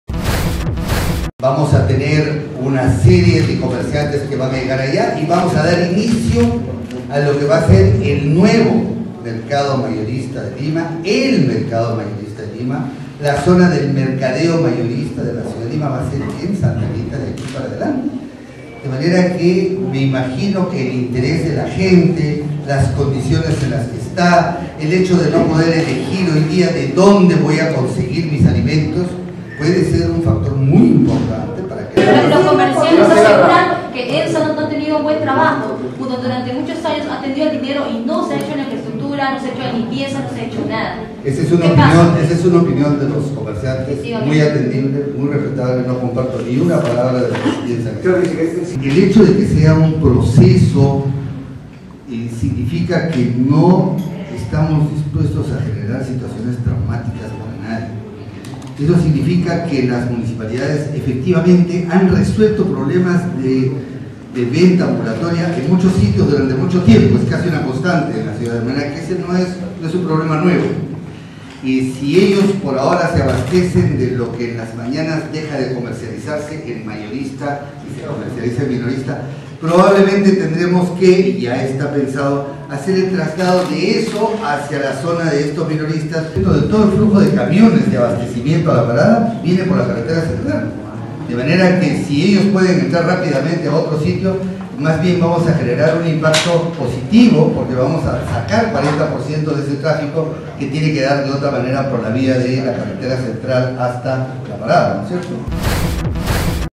ALFA NOTICIAS- EMSSA RESPALDA CAMBIO DE SANTA ANITA POR SER POSITIVO
EL GERENTE DE EMSSA RESPALDA EL CAMBIO AL MERCADO SANTA ANITA